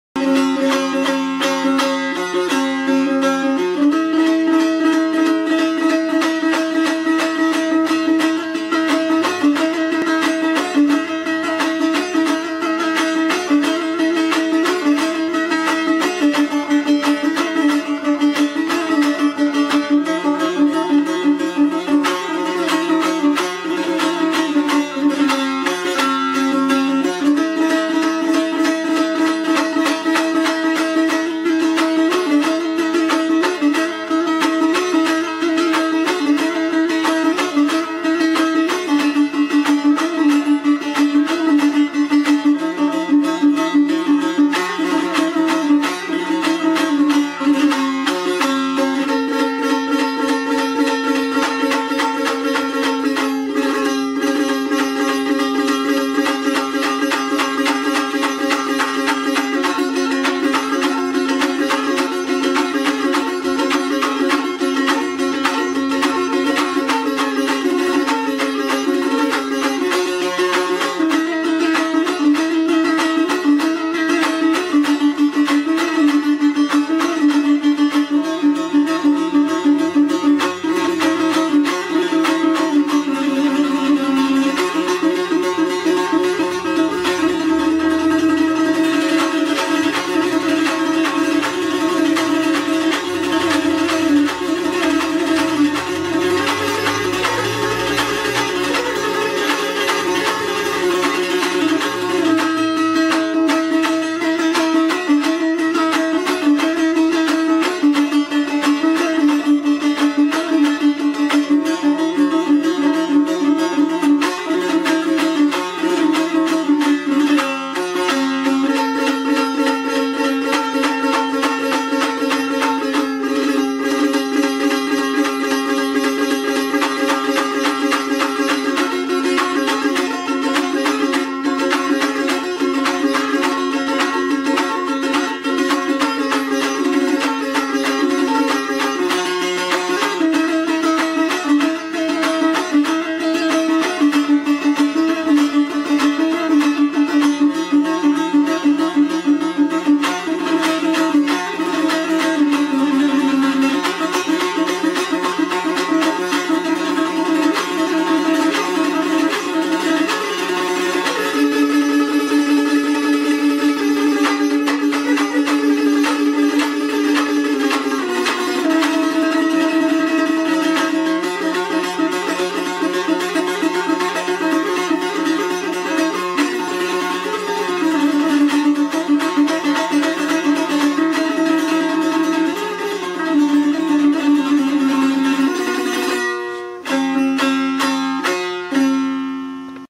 ابراهیم نادمی طرقه تک نوازی دوتار
سلام . امروز با نواختن موزیکی زیبا با ساز دوتار در خدمت همه ی شما عزیزان هستیم . سابسکرایب و کامنت و لایک هم فراموش نشه . این کار شما خیلی مارو کمک میکنه .